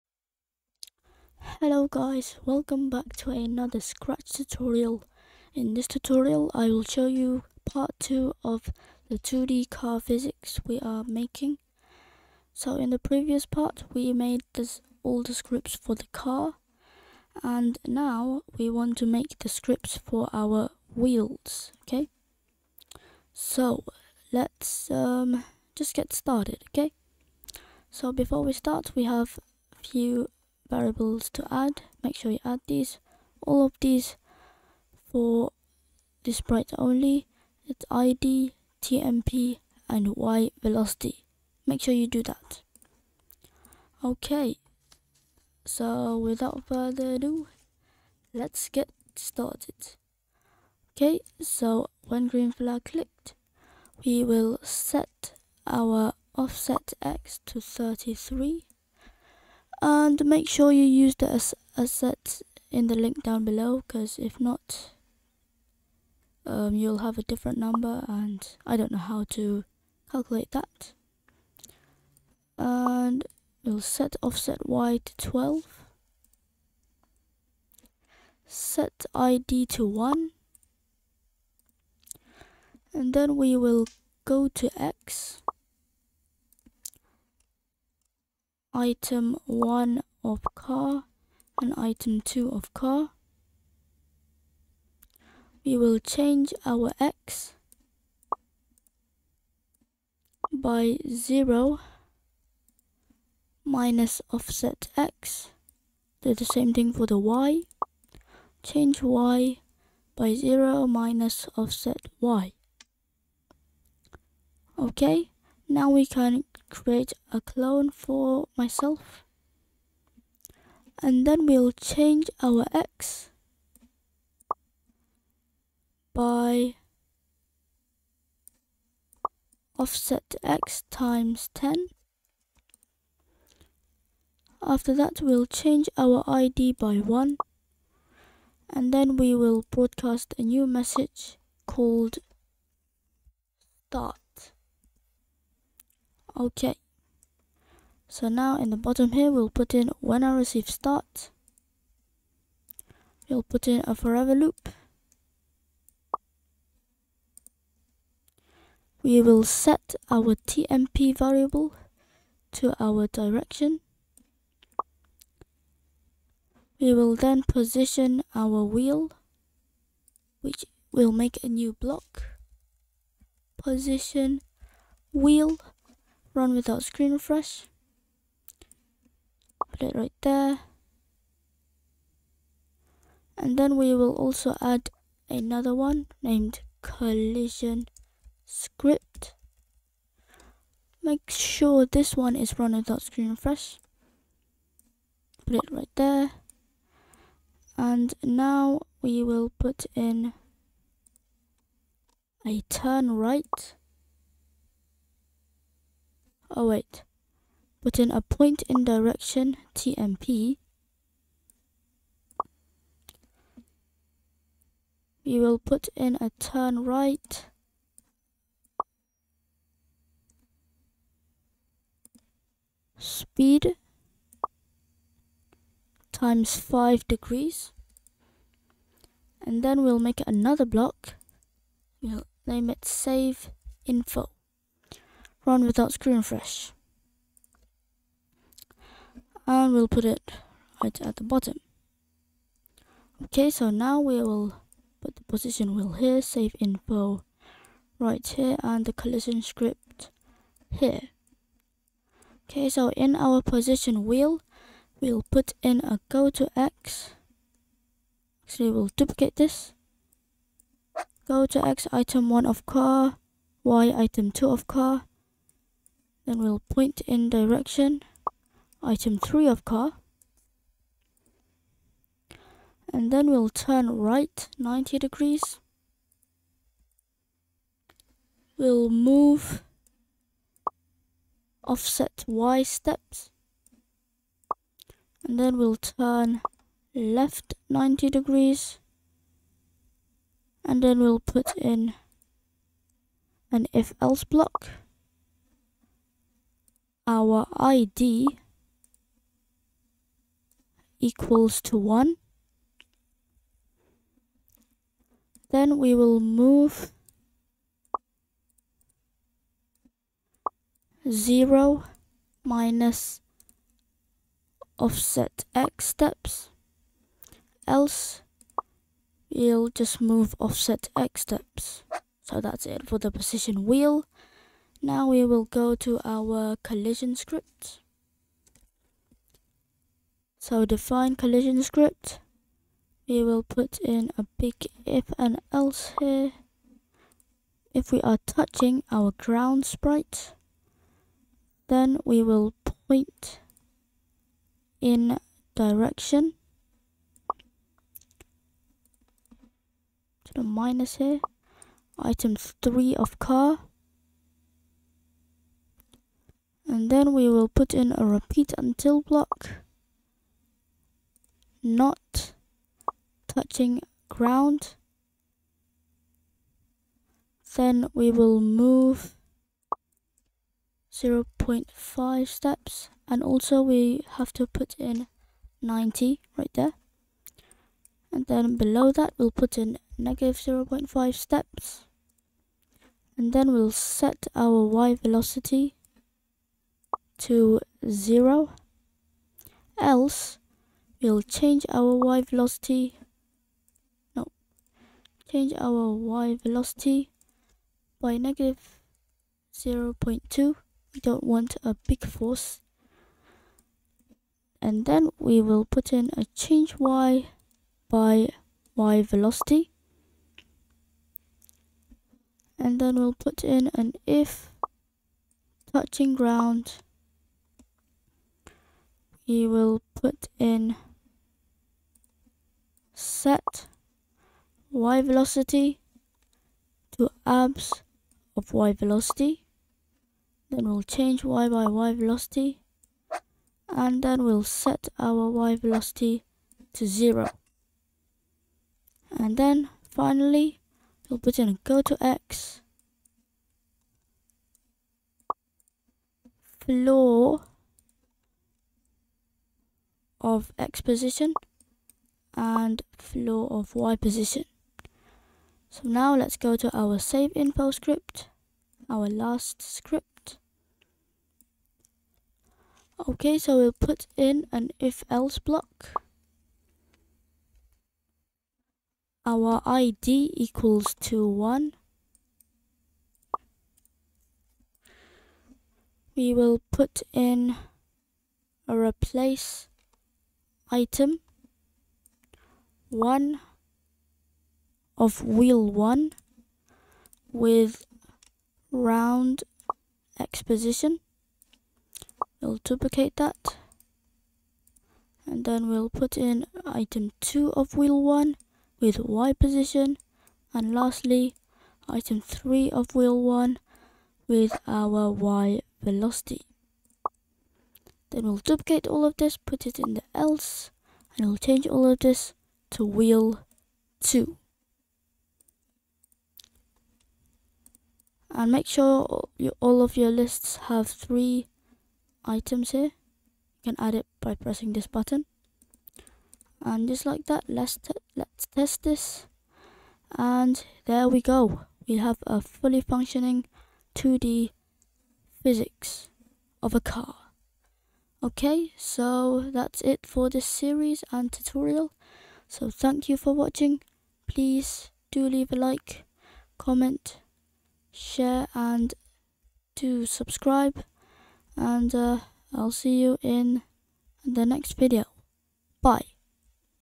How to Make 2D Car Physics in Scratch (Part 2)
In this video, I will show you how to make 2D Car physics in Scratch!